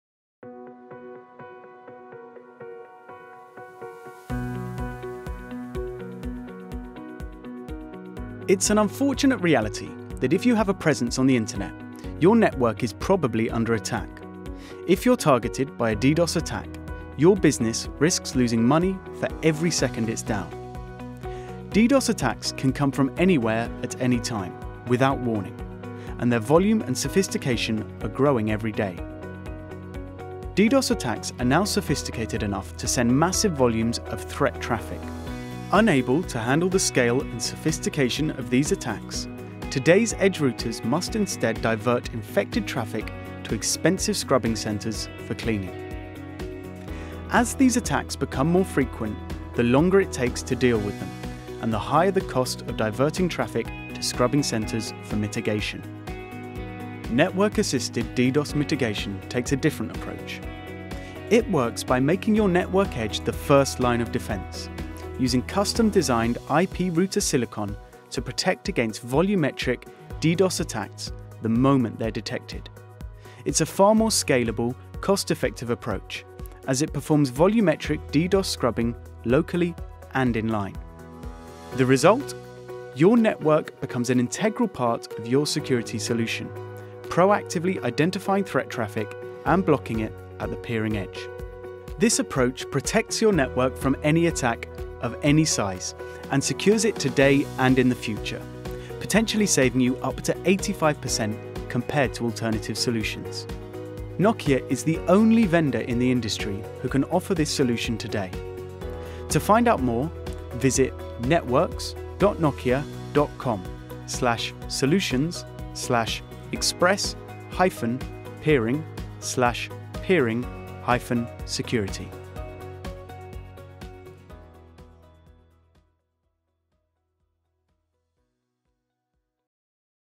Peering series pt3: Network-assisted DDoS mitigation
Counter high volume, sophisticated DDoS attacks with insight-driven DDoS mitigation to proactively identify and block threats at your peering edge and at a lower cost compared to existing solutions.